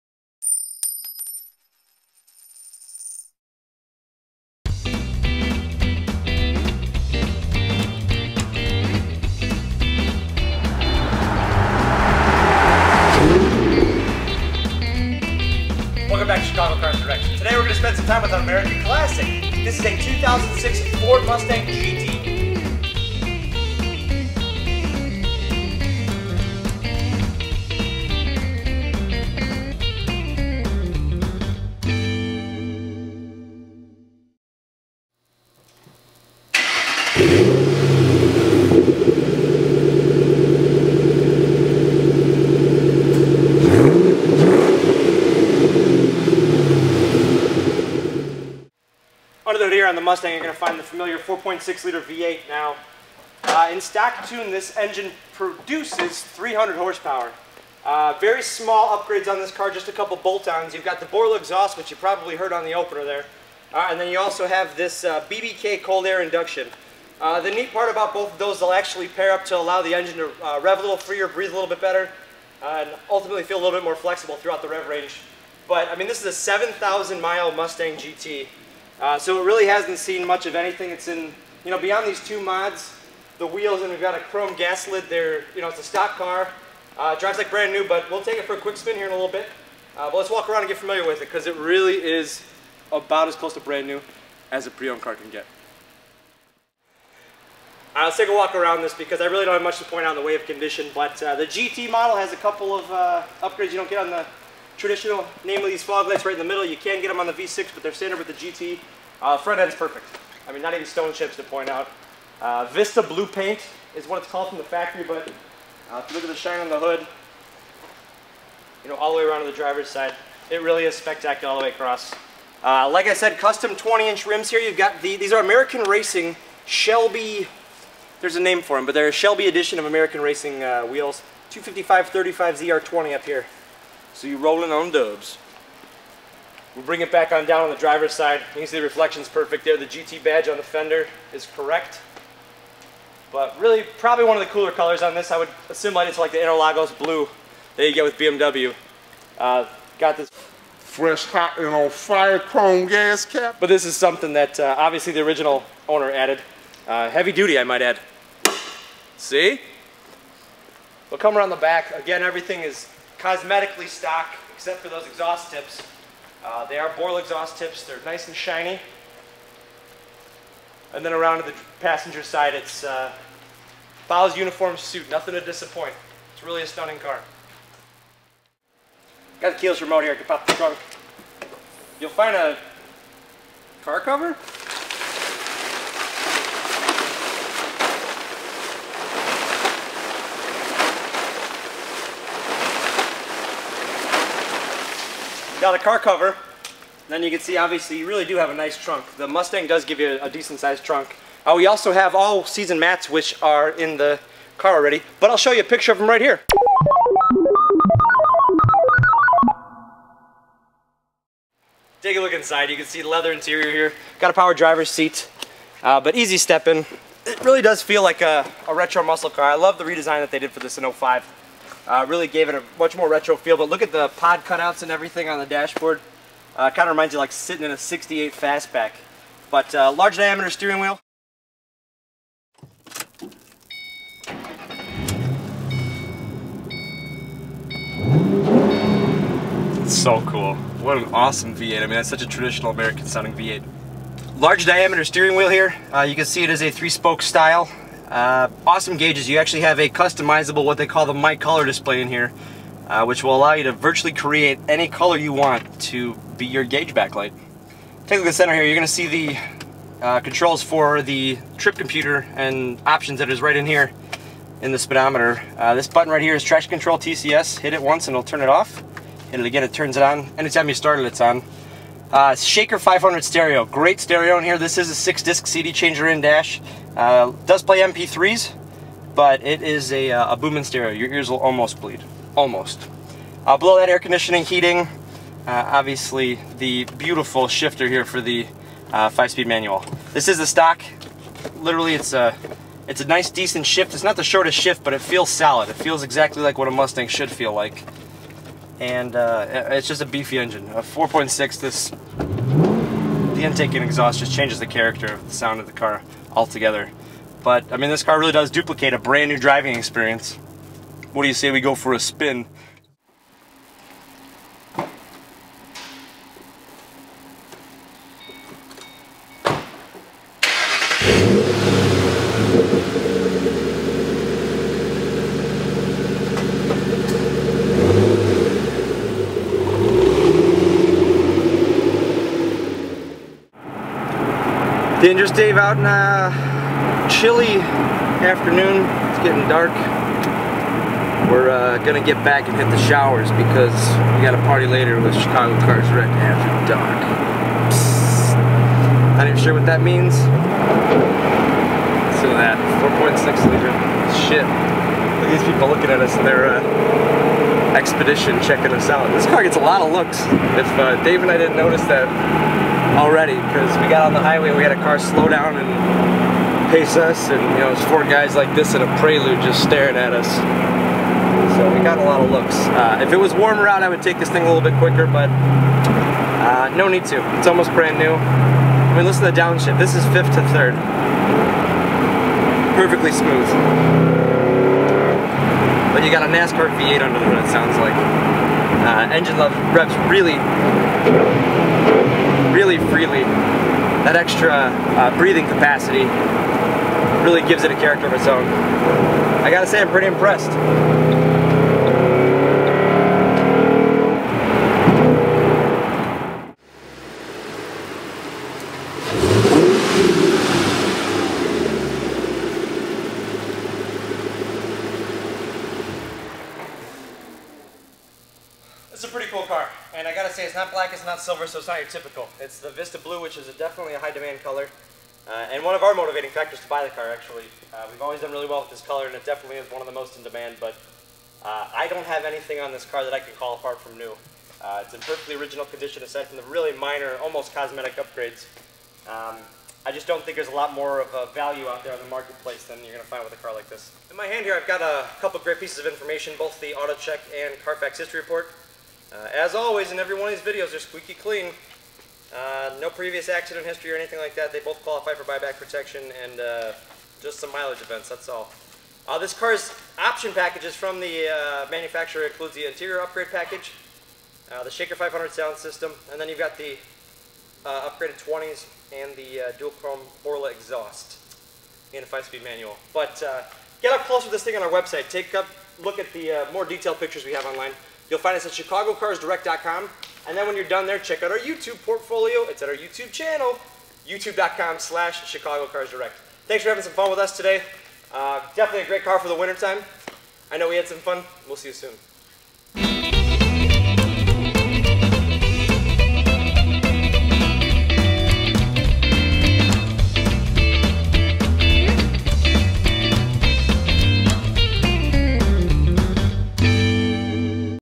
Ford Mustang GT--Chicago Cars Direct HD
Test drive and walk around of a 2006 Ford Mustang GT from Chicago Cars Direct.